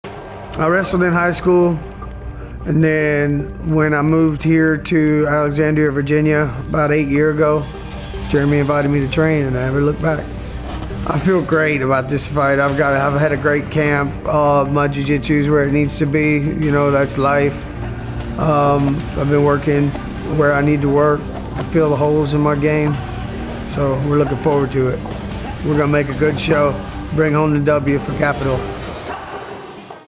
Interview with Jonathan Vinyard Capital MMA Fighter Strike Off 6
Strike Off MMA Eaglebank Arena October 3rd. VA ranked fighter at 135LB.

Comcast Sports Network TV Channel